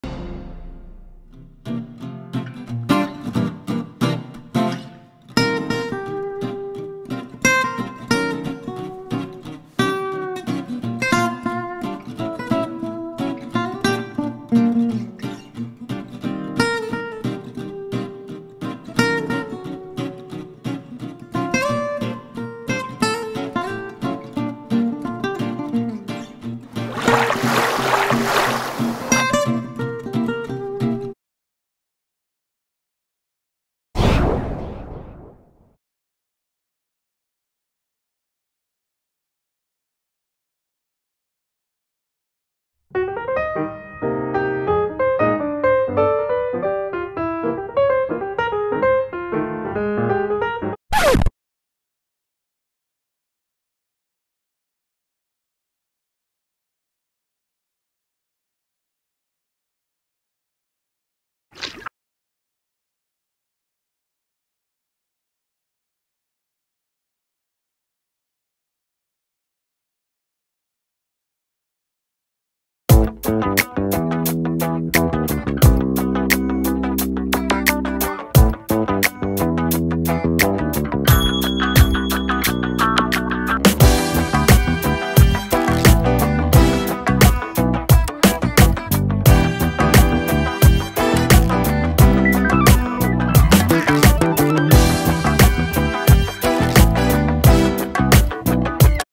Da Widgets- Ep1 & Ep2
The first and second episode of Da Widgets, is finally out! Remember, new episodes on Sundays :D
Credits: Me(Obviously), My brothers, PowerPoint, Imovie, and Logic Pro.